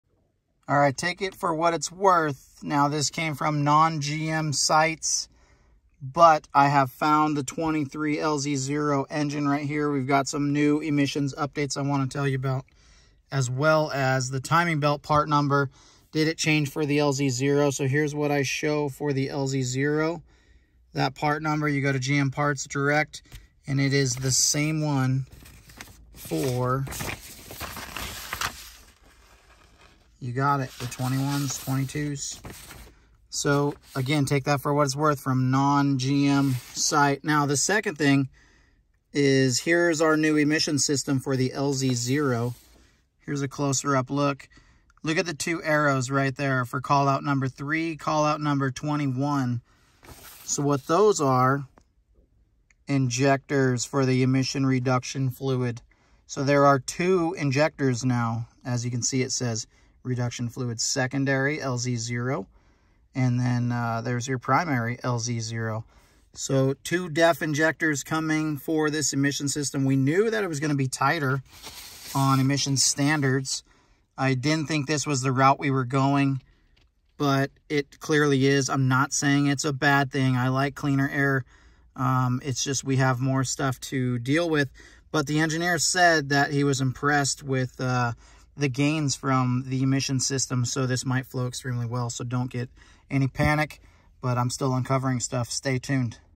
Duramax 3.0 LZ0 getting 2 def injectors in emissions system - oil pump belt is same as lm2
LZ0 is the upcoming replacement for the LM2 3.0 Duramax. With that is new emissions and as found here, 2 DEF injectors - That's not bad though, I know you probably immediately thought it would be double the DEF usage. Hear me out, 2 smaller systems therefore becoming just a mist and reducing NOx at a quicker rate since you aren't soaking the SCR?! Your support is appreciated as it helps the channel to continue to test products!